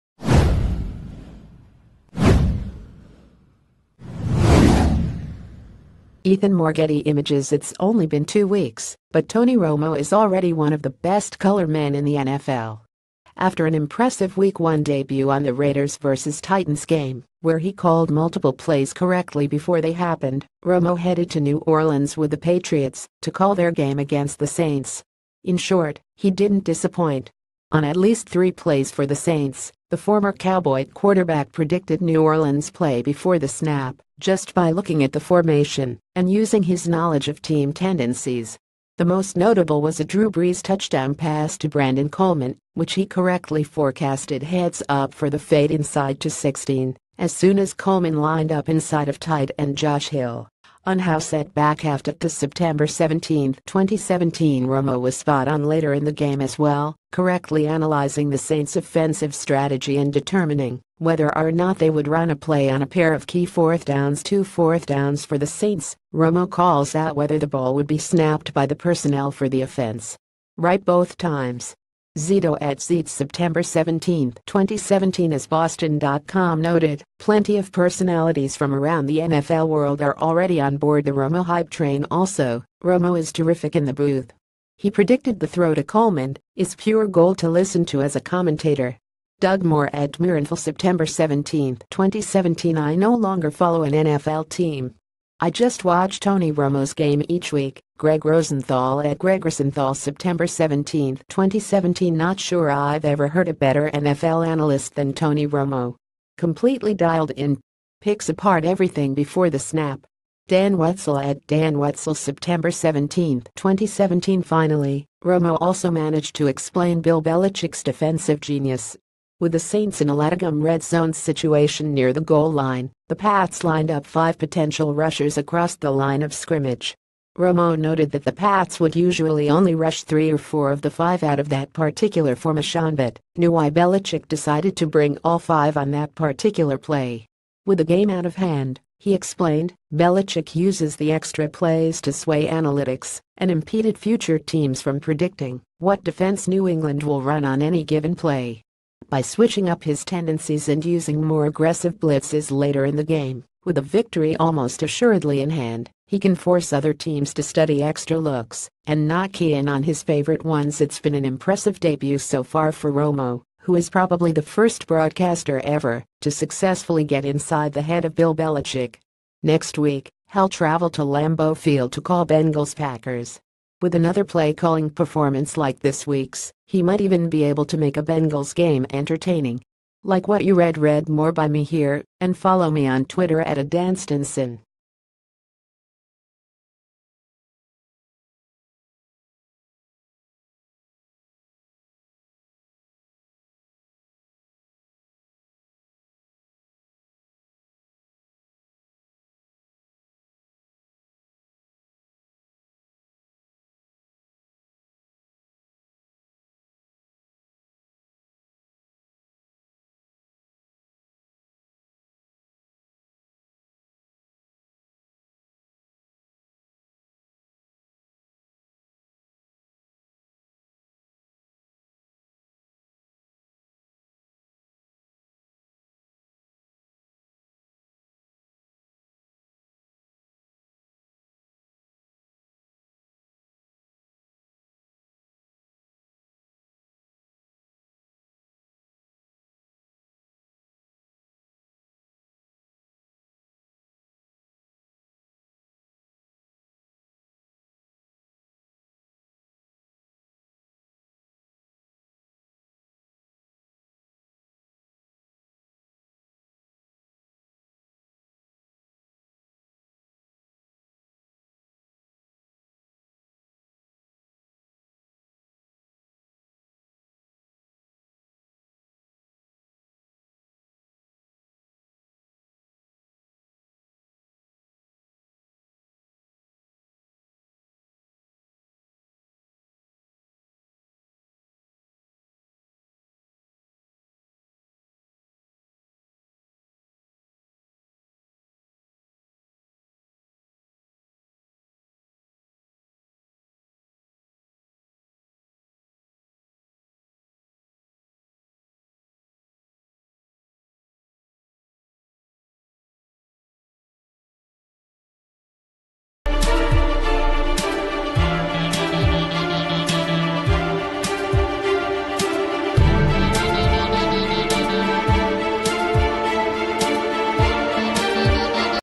Breaking News | Tony romo keeps predicting plays, explains genius of bill belichick
Tony romo keeps predicting plays, explains genius of bill belichick

Ethan Moore Getty Images Its only been two weeks, but Tony Romo is already one of the best color men...